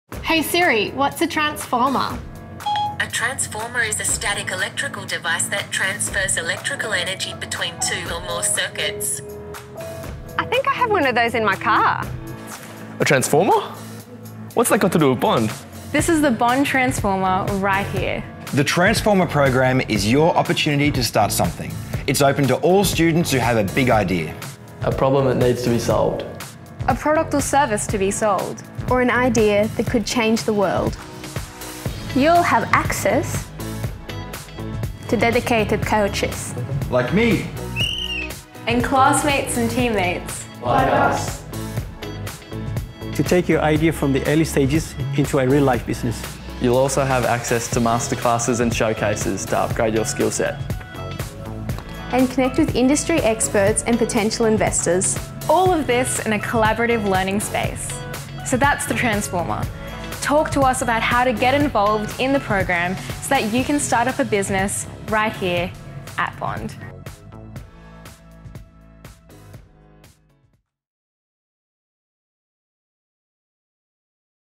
What is Transformer?
Is it a robot? Is it in your car? No, Transformer is an Australian-first entrepreneurship program, right here at Bond University! Watch Transformer students break down what Transformer is, what students will gain from the fee-free, extracurricular program, and how by being involved, you could create your dream job while you’re still at university.